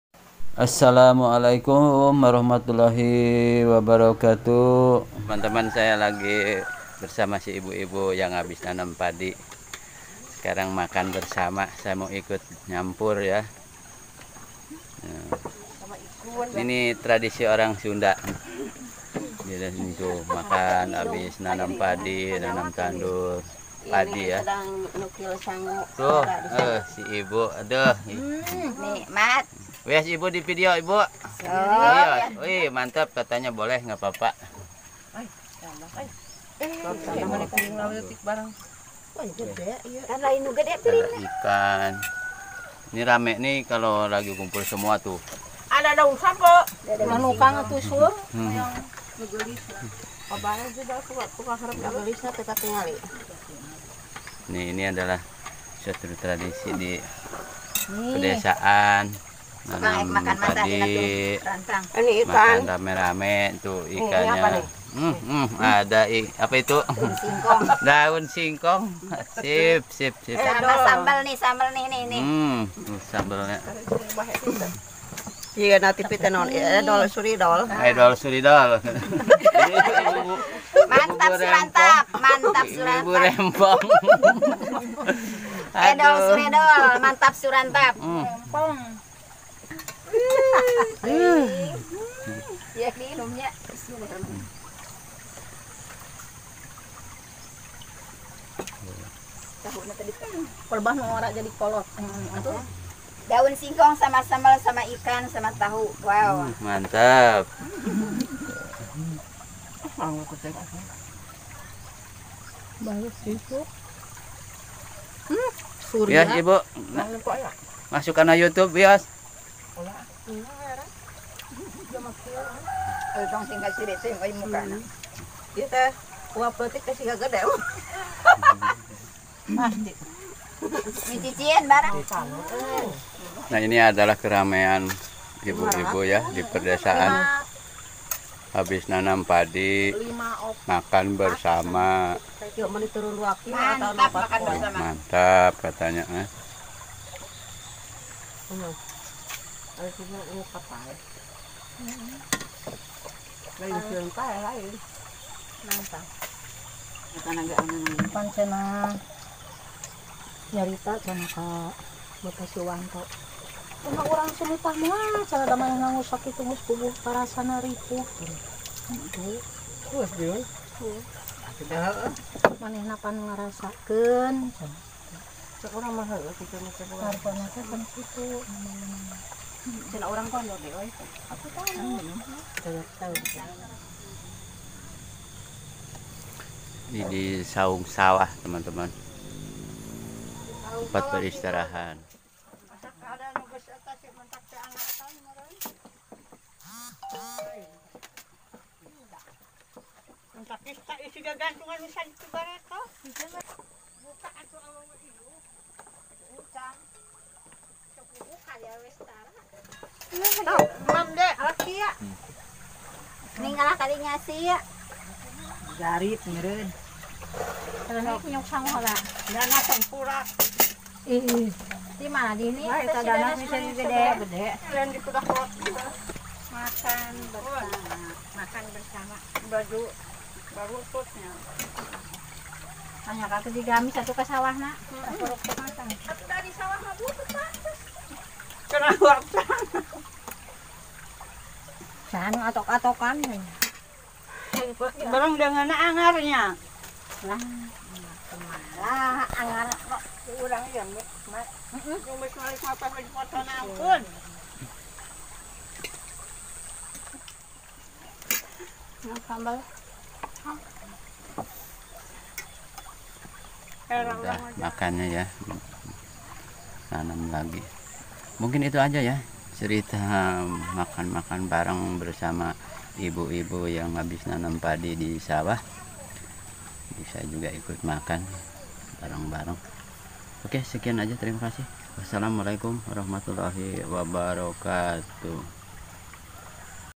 Keseruan Makan Bersama di Saung Sawah
Keseruan Makan Bersama di Saung Sawah suatu kegiatan makan bareng di musim Tanam Padi , ini biasanya makan dipinggiran sawah ataupun bisa juga di Saung Sawah sambil menikmati keindahan Alam Pedesaan yang damai , asri berbagi beraneka  makanan dan lauk pauknya.

Mungkin ini juga merupakan adat turun temurun dari pendahulunya sehingga kebersamaan ini masih terjaga sampai Sekarang, Demikian terimakaih